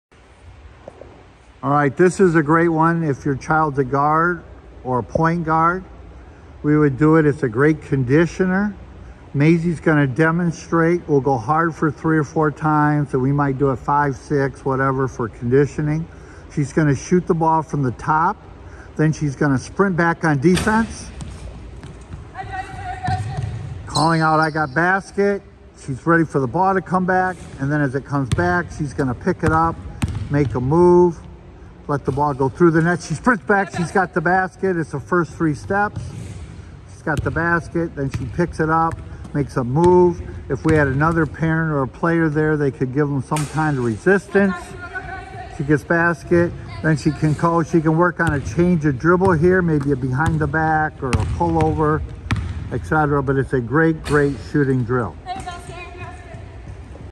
Mastering the Art of Shooting and Transition Defense || OMG - Robert Taylor
In this captivating video, we delve into the mesmerizing world of basketball as we explore the mastery of shooting and defense by a remarkable Point Guard. Prepare to be amazed as we witness the phenomenal I.C.I techniques (inspired by the wisdom of The old man in the gym) in action, taking your shooting skills to unimaginable heights and enhancing your defensive prowess like never before! In this thrilling display of athleticism and technique, our skilled Point Guard demonstrates how the T.L.C (Turned Lined and Comfortable) approach can revolutionize your shooting game. Using a cutting-edge shooting machine, she showcases her exceptional ability to drain baskets from various positions on the court. Witness the precision, accuracy, and fluidity as she effortlessly releases the ball into the net, as we watch with awe. But it doesn't stop there! Our dynamic Point Guard proves that versatility is the key to success on the basketball court. After sinking those impeccable shots, she seamlessly transitions into defensive mode, sprinting back to thwart any opponent's advance. Experience the intensity as she showcases her agility, agility, and quick reflexes, ensuring her team's has defensive basketball coverage while exuding an aura of unmatched confidence. To top it all off, our Point Guard showcases her incredible dribbling skills as she then sprints upcourt with the basketball, artfully maneuvering past defenders with finesse and grace. Witness the mastery of ball control and speed as she electrifies the court with lightning-fast crossovers, leaving defenders in her wake. Prepare to be inspired by this incredible display of skill and determination! Join us on this unforgettable journey as we witness the convergence of shooting finesse, defensive brilliance, and unrivaled ball handling. Learn from the best and unlock your true potential as a Point Guard. Mastering the Art of Shooting and Defense is no longer a dream – it's a reality waiting for you to embrace! Don't miss out on this opportunity to elevate your game to new heights.

Together, let's revolutionize the way you play the game!

00:00 Introduction
00:14 Drill Demonstration
00:31 PG vs Shooting Gun
00:43 PG Move on the Move